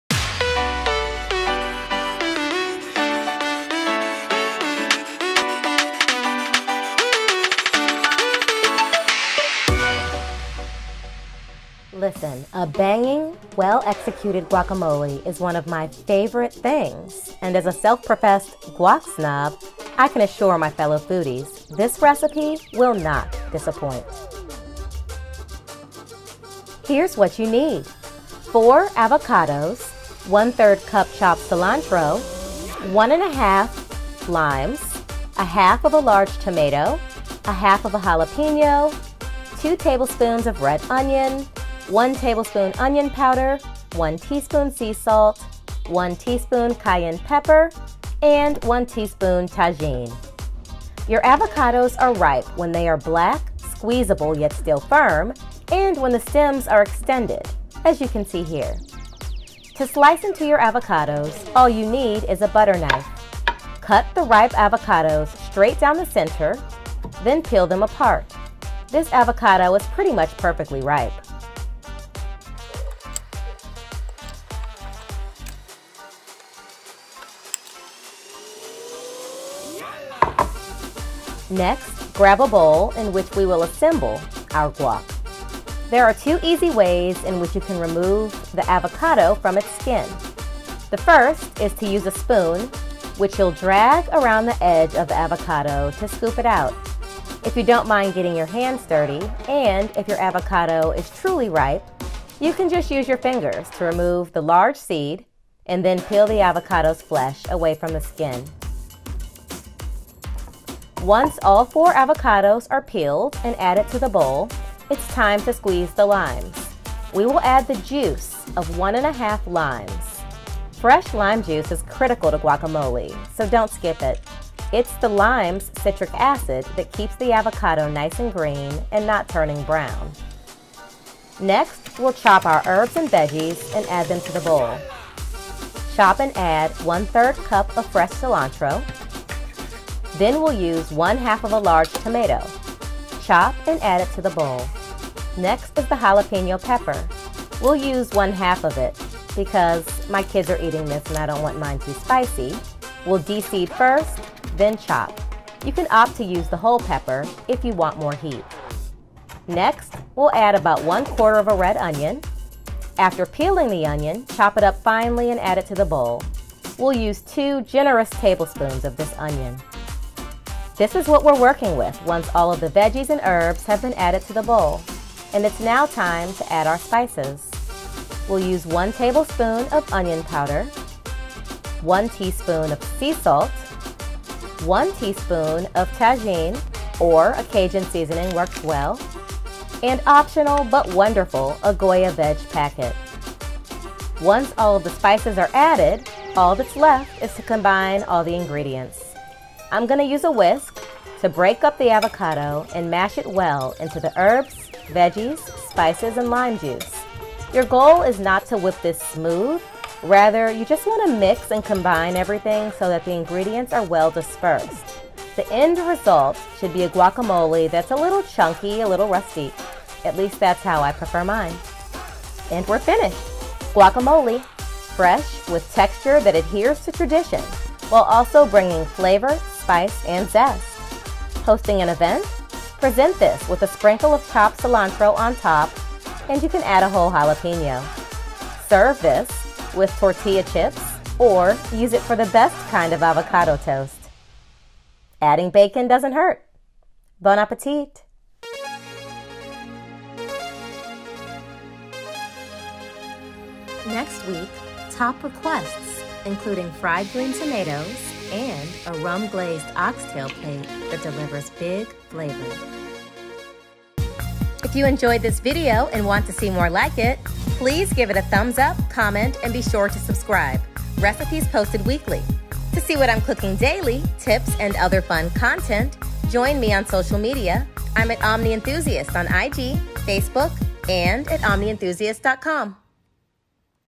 PERFECT GUACAMOLE W/ TAJIN! CREAMY + CHUNKY + ZESTY
Fresh guacamole with all of the right flavors is one of my favorite things!

I’ve eaten more than my fair share in amazing Mexican restaurants and also enjoyed it in travels throughout Mexico.

As a self-professed 'guac snob,' I assure you, my fellow foodie brethren, that my recipe will not disappoint.

If you’re a fan of the tableside-style guac you get in authentic Mexican restaurants and appreciate robust flavor, kick and texture — this recipe is right for you.

---INGREDIENTS---
4 Hass avocados
⅓ cup chopped cilantro
1.5 limes (juice of)
½ large tomato, chopped
½ jalapeno chopped
2 tbsp red onion, chopped
1 tbsp onion powder
1 tsp sea salt
1 tsp tajin
1 tsp cayenne pepper
Optional: 1 packet of Goya vegetable seasoning

---INSTRUCTIONS---

Cut avocados down the middle and remove the large seed.

Peel or spoon the flesh from each avocado away from its skin and into a large bowl.
Cut the two limes in half. Squeeze the juice from one and a half limes into the bowl and over the avocado.  (A hand-held citrus squeezer works great for this.)

De-seed one half of a jalapeno and finely chop. Chop tomato and red onion. Add to bowl.

Add the seasonings: 1 tbsp onion powder, 1 tsp sea salt, 1 tsp tajin (cajun seasoning is an ok substitute) and 1 tsp cayenne pepper.

Use a whisk to mash everything together. Avocados should be mashed, but consistency should be slightly chunky.

Chill then serve with tortilla chips, on toast, in a rice bowl or simply on a spoon!